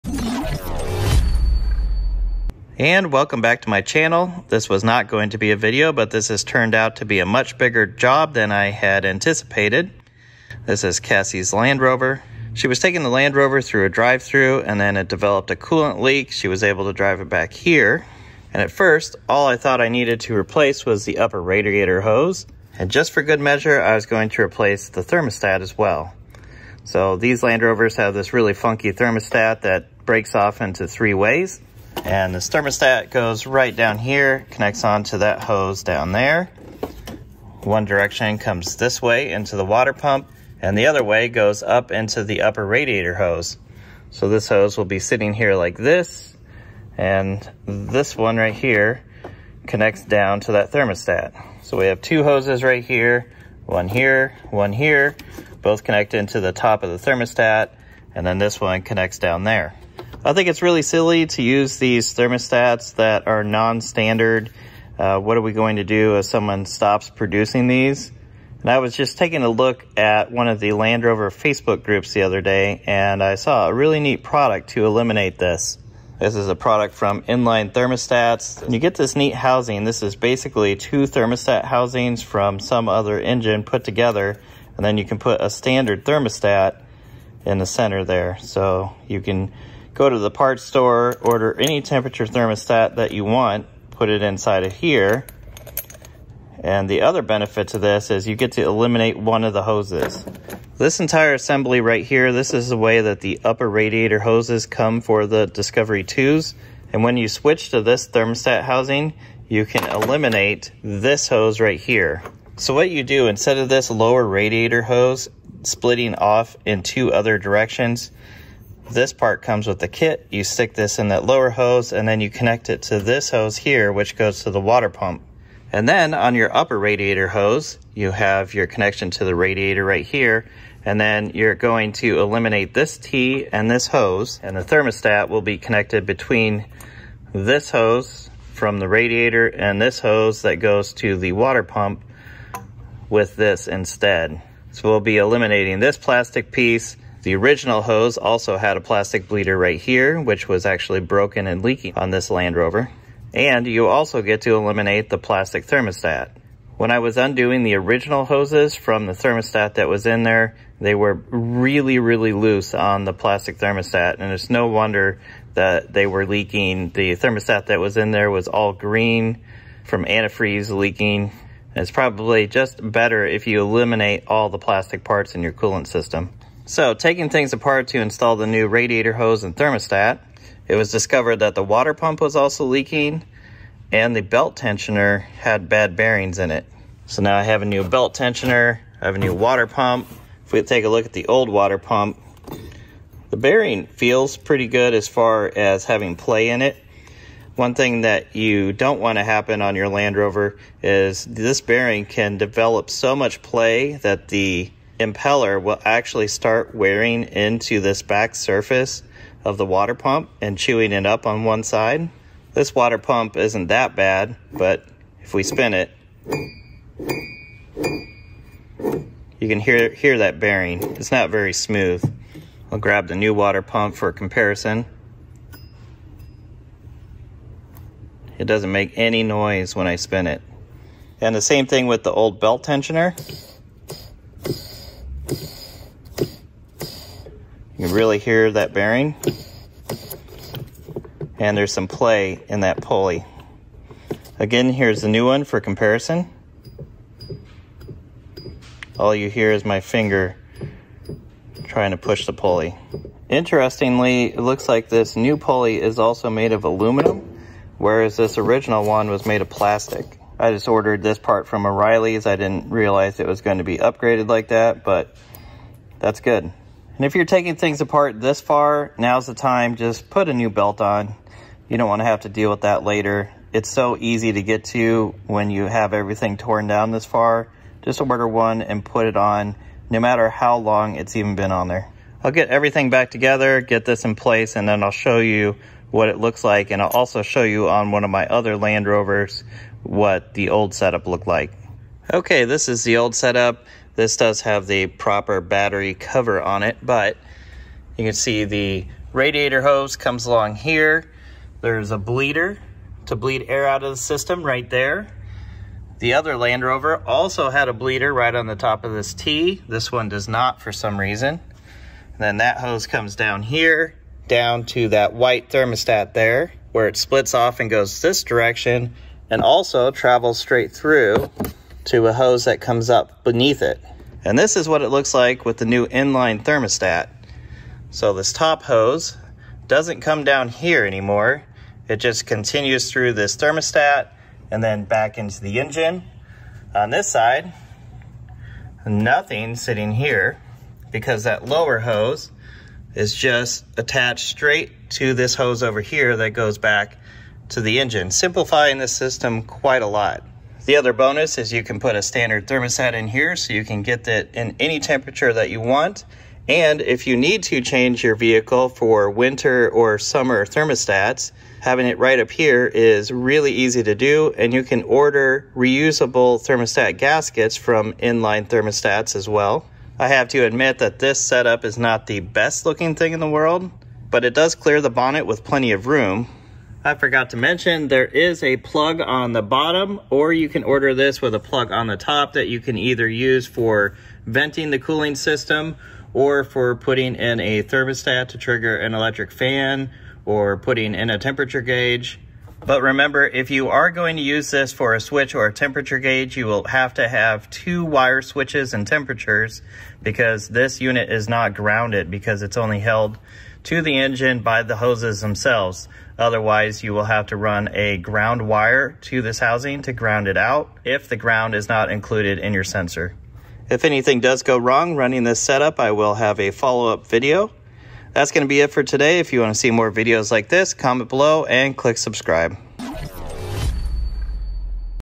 Changing to an Inline Thermostat on Land Rover Discovery II
Today I install a new thermostat from Inline Thermostat into Cassie's Land Rover Discovery II.